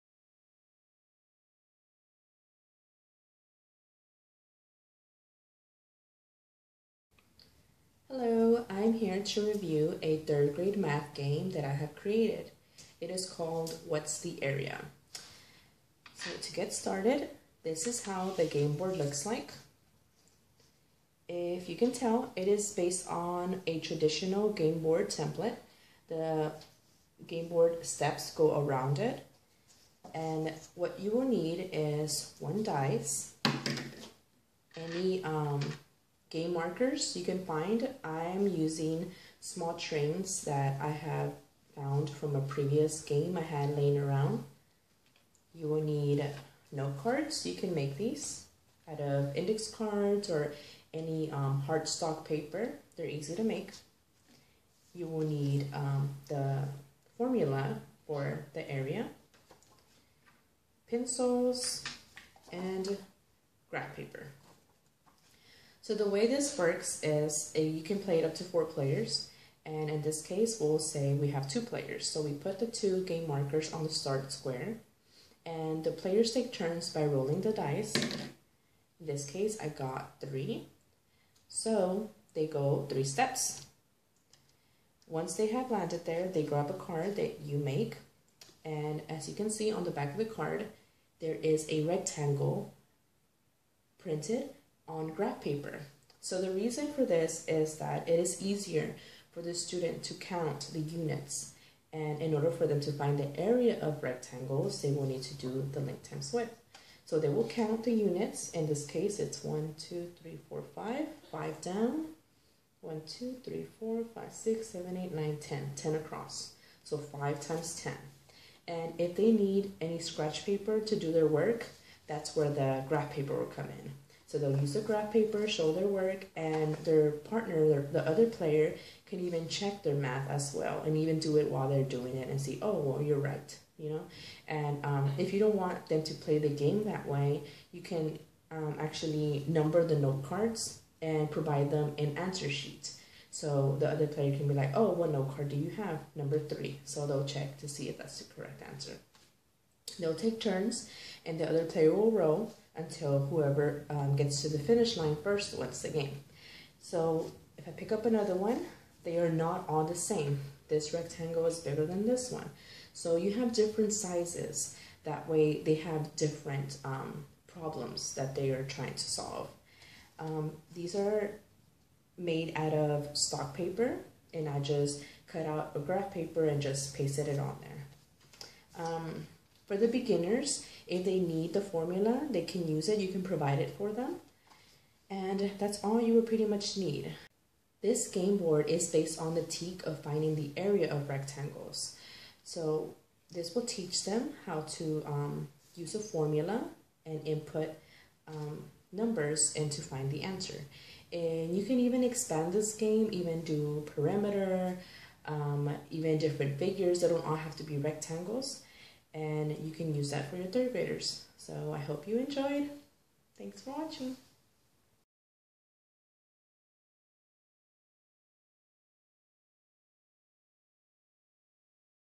Math Game #1 for 3rd graders
This is a video showing a homemade math game on finding the area of rectangles. This game is designed for 3rd graders.